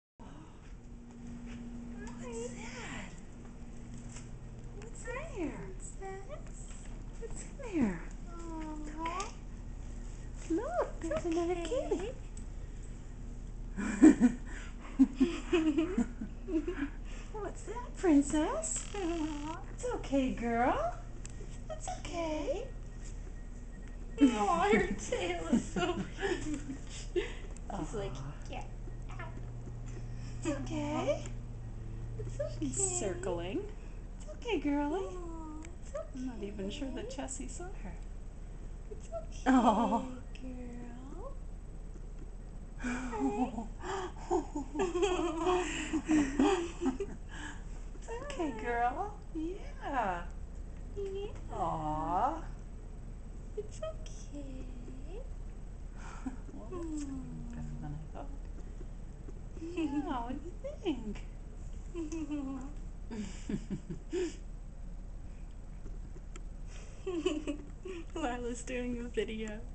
Chessie and Princess meeting for the first time
This is a video of when Chessie and Princess first met each other. After 4 years, they still don't get along.